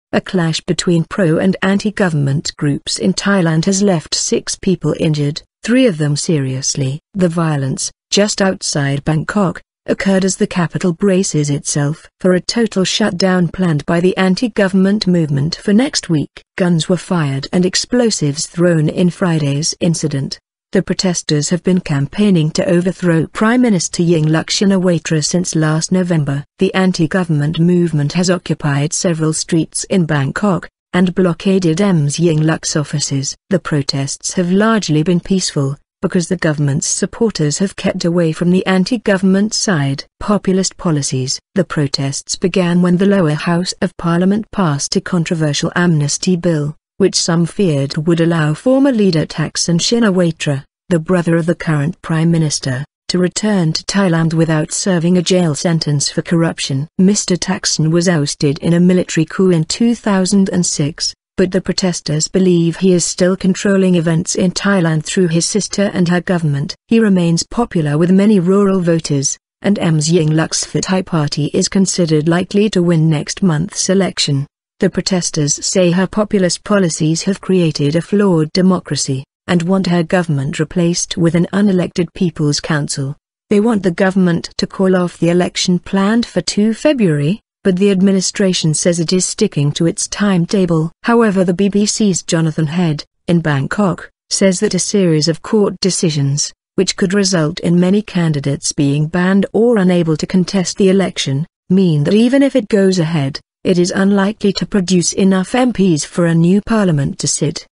6 People Injured At Thailand Clashes
A clash between pro- and anti-government groups in Thailand has left six people injured, three of them seriously.

The violence, just outside Bangkok, occurred as the capital braces itself for a total shutdown planned by the anti-government movement for next week.

Guns were fired and explosives thrown in Friday's incident.

The protesters have been campaigning to overthrow Prime Minister Yingluck Shinawatra since last November.

The anti-government movement has occupied several streets in Bangkok, and blockaded Ms Yingluck's offices.

The protests have largely been peaceful, because the government's supporters have kept away from the anti-government side.
Populist policies

The protests began when the lower house of parliament passed a controversial amnesty bill, which some feared would allow former leader Thaksin Shinawatra - the brother of the current prime minister - to return to Thailand without serving a jail sentence for corruption.

Mr Thaksin was ousted in a military coup in 2006, but the protesters believe he is still controlling events in Thailand through his sister and her government.

He remains popular with many rural voters, and Ms Yingluck's Pheu Thai party is considered likely to win next month's election.

The protesters say her populist policies have created a flawed democracy, and want her government replaced with an unelected "People's Council".

They want the government to call off the election planned for 2 February, but the administration says it is sticking to its timetable.

However the BBC's Jonathan Head, in Bangkok, says that a series of court decisions - which could result in many candidates being banned or unable to contest the election - mean that even if it goes ahead, it is unlikely to produce enough MPs for a new parliament to sit.